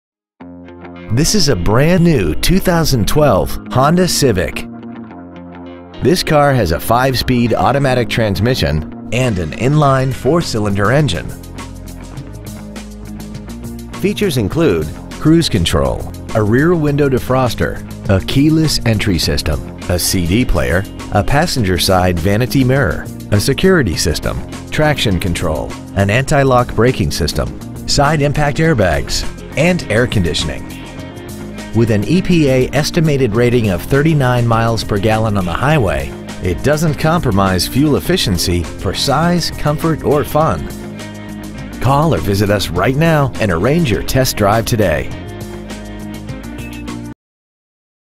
2012 Honda Civic Richardson TX 75080
Year : 2012
 Make : Honda
 Model : Civic
 Engine : 1.8L I-4 cyl
 Trans . : 5-Speed Automatic
 Exterior : Crystal Black Pearl
 Miles : 10
 Interior : Gray

 2012 Honda Civic Richardson TX